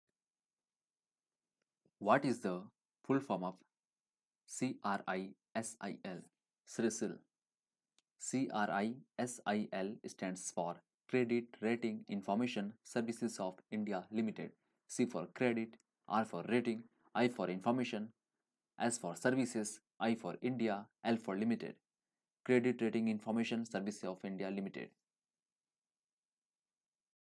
Full form of CRISIL | CRISIL ka full form kya hai | Free Learn University
Welcome to free learn university. In this vedio you will know the full form of CRISIL.